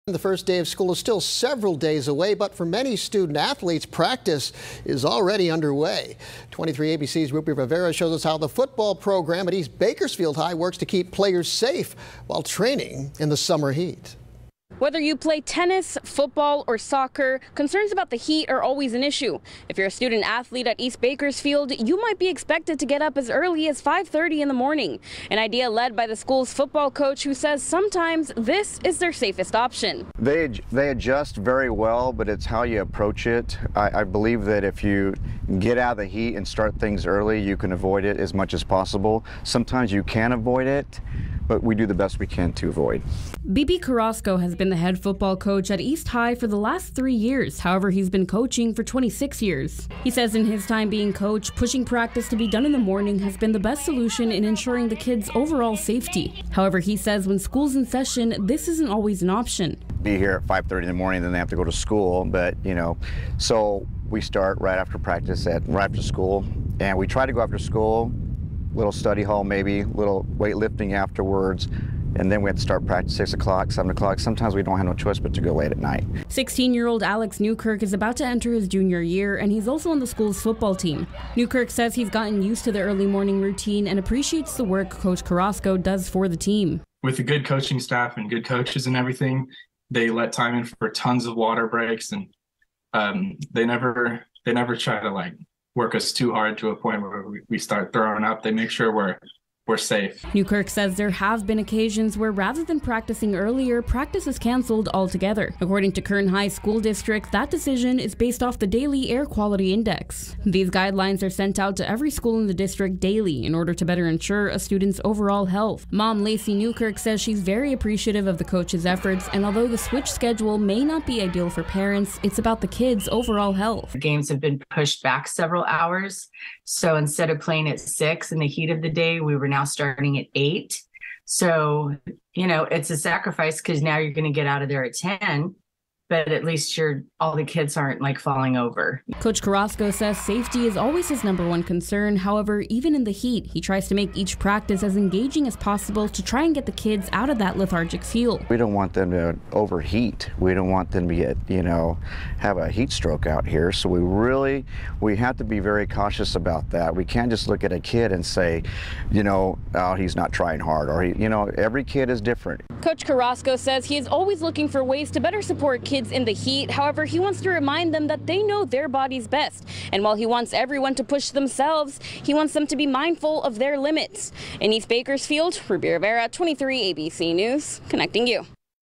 Student athletes shift practice, game times to avoid Bakersfield heat
The first day of school is still several days away, but for many student athletes, including the Blades of East Bakersfield High School, practice is already underway. With the summer heat still on, the Blades football program is working to keep players safe. 23ABC's Ruby Rivera visited East High to find out more.